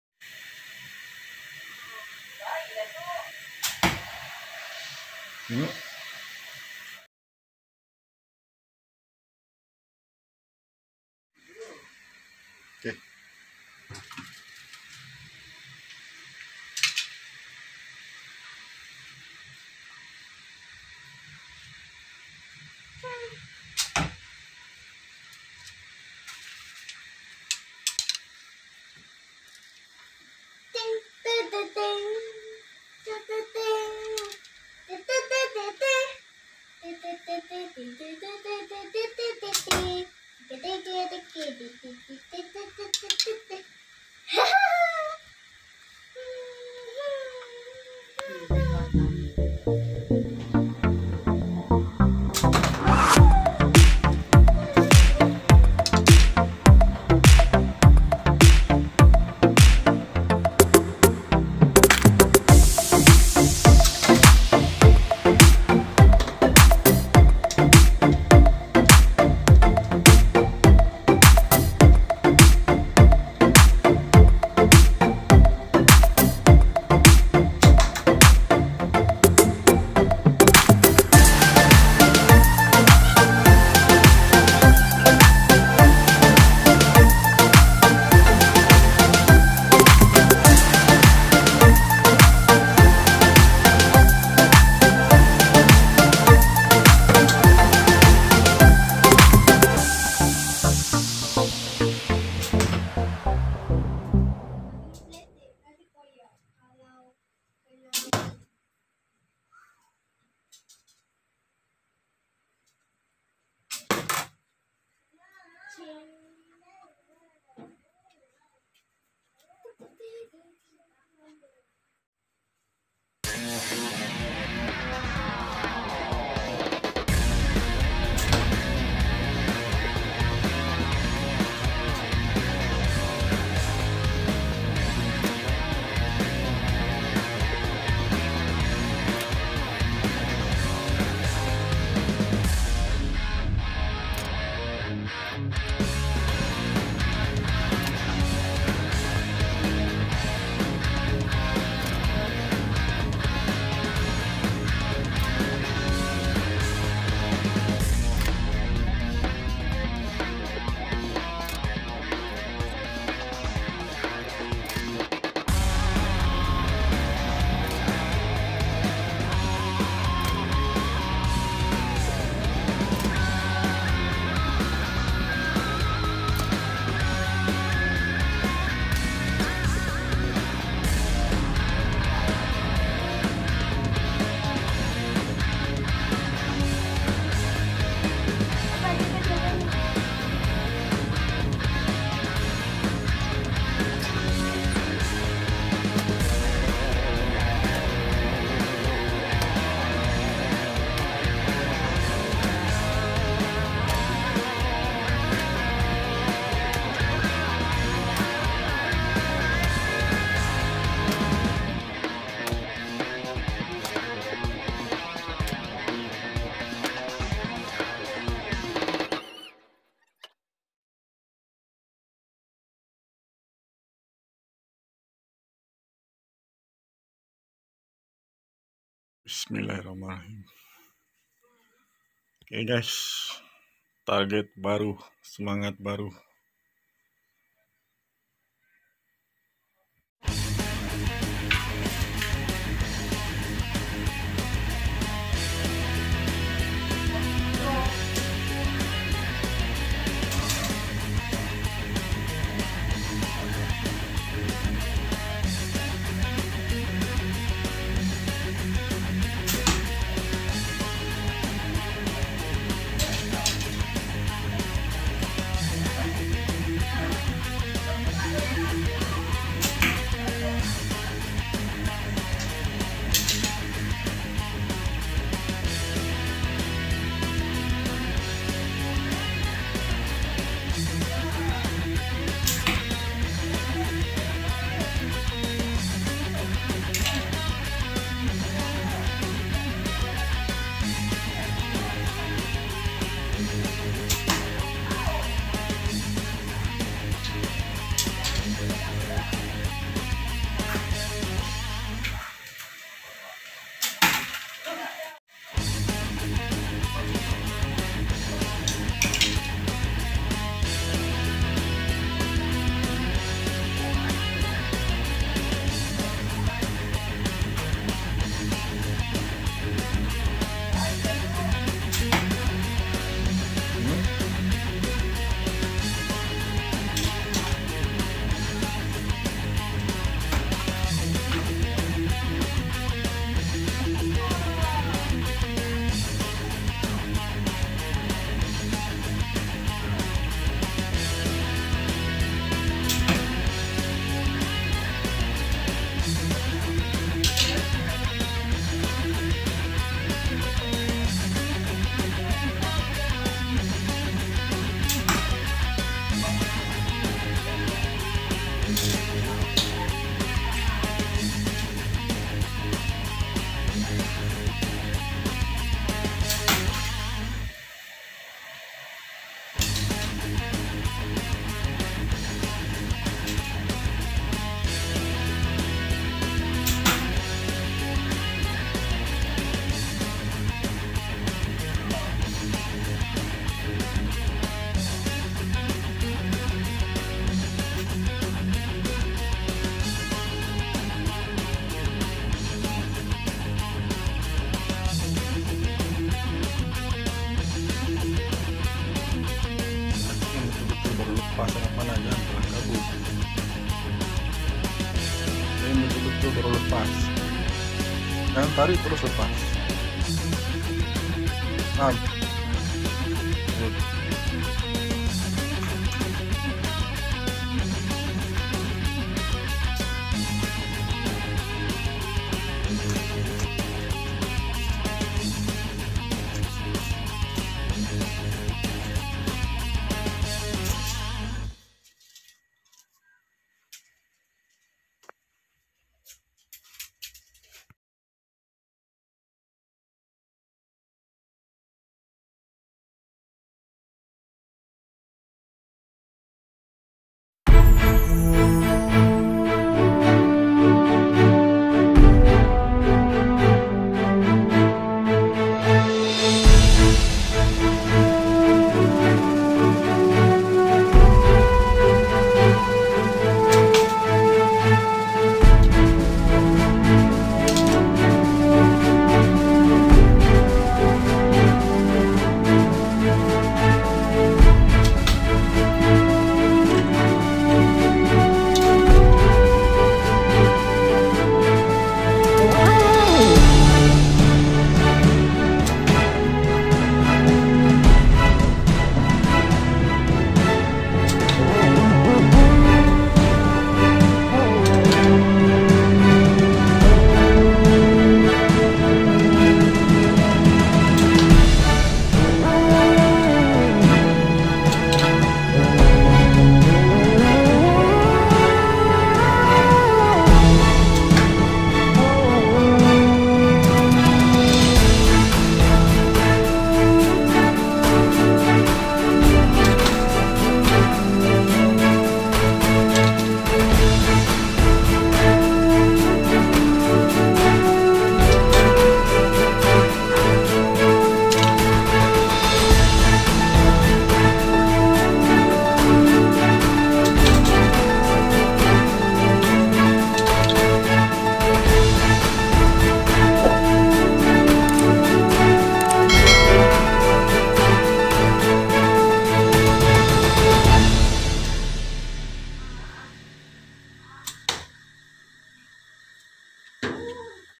Aktiviti semasa MCO||target baru semangat baru||jom memanah
Aktiviti semasa mco,anak2 gembira dapat target baru..puas hati katanya memanah.. yg lama asyik tersasar.. kini tidak lagi.. latihan demi latihan..teknik2 di ajarkan apa yg ada.. sama2 sebenarnya belajar..